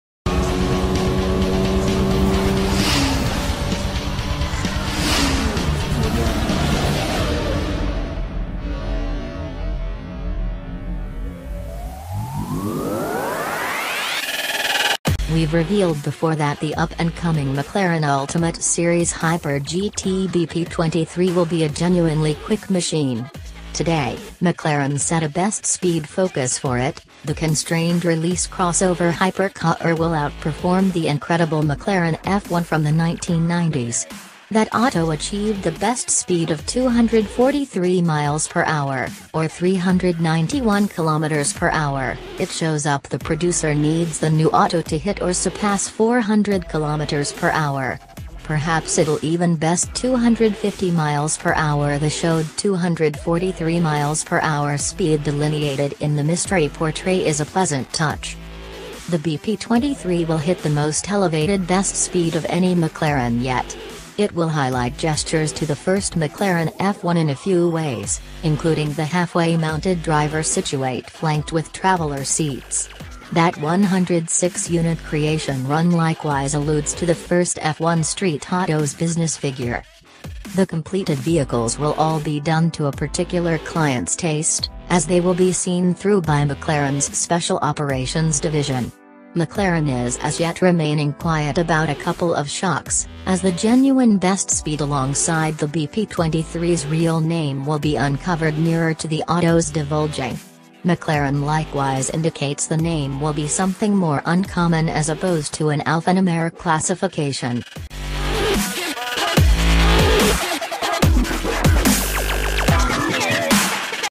New McLaren hybrid hypercar will be company's fastest ever
We've revealed before that the up and coming McLaren Ultimate Series "Hyper-GT" BP23 will be a genuinely quick machine. Today, McLaren set a best speed focus for it: The constrained release crossover hypercar will outperform the incredible McLaren F1 from the 1990s. That auto achieved the best speed of 243 mph, or 391 km/h; it shows up the producer needs the new auto to hit or surpass 400 km/h. Perhaps it'll even best 250 mph. The showed 243 mph speed delineated in the mystery portray is a pleasant touch.

The BP23 will hit the most elevated best speed of any McLaren yet. It will highlight gestures to the first McLaren F1 in a few ways, including the halfway mounted driver situate flanked with traveler seats. That 106-unit creation run likewise alludes to the first F1 street auto's business figure.

The completed vehicles will all be done to a particular client's taste, as they will be seen through by McLaren's Special Operations division. McLaren is as yet remaining quiet about a couple of shocks, as the genuine best speed alongside the BP23's real name will be uncovered nearer to the auto's divulging. McLaren likewise indicates the name will be something more uncommon "as opposed to an alphanumeric classification."